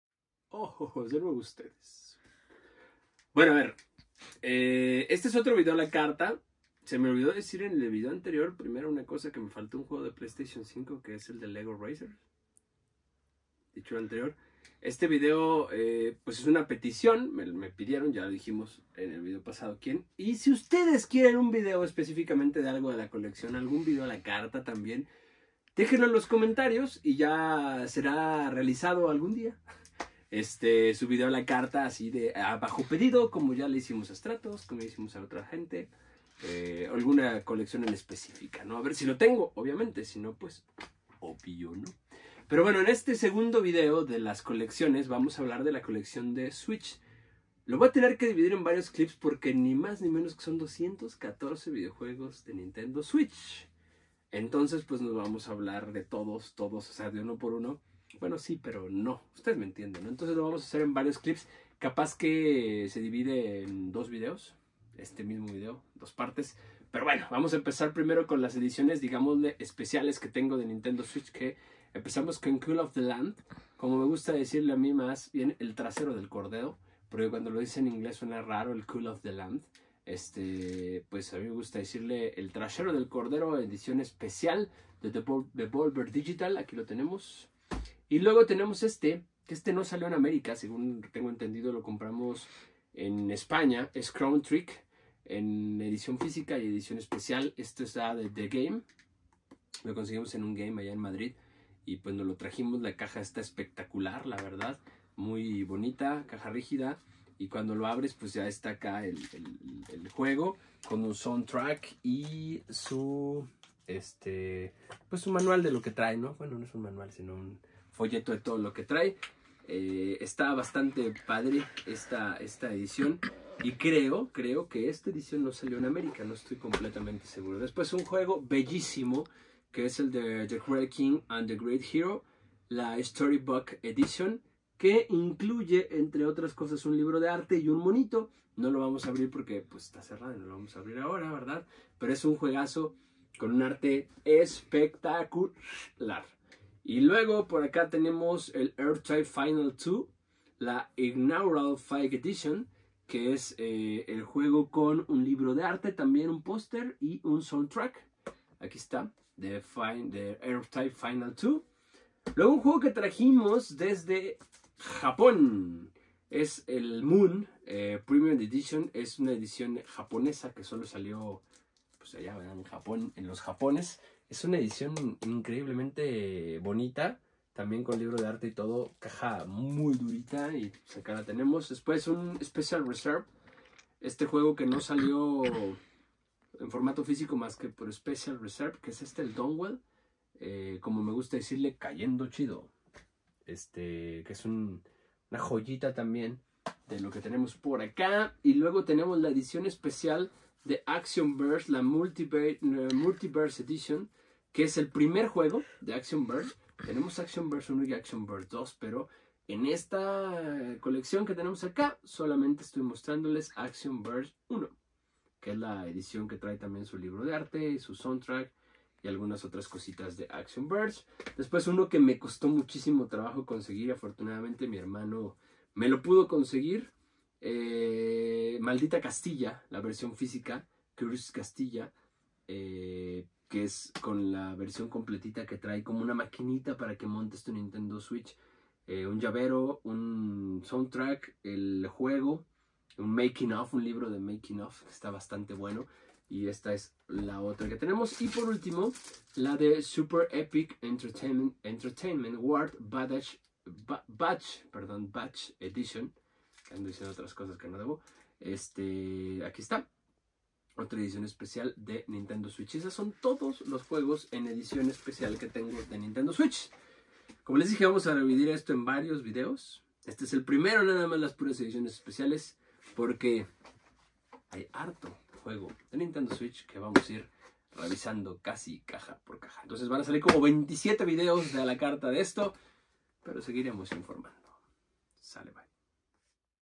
A LA CARTA 006 - La colección de Nintendo Switch (Parte 1)
La primera parte de la colección de Nintendo Switch.

Video solicitado por ​⁠ @Luxo_SvN

Entra al grupo de LOS COLECCIONISTAS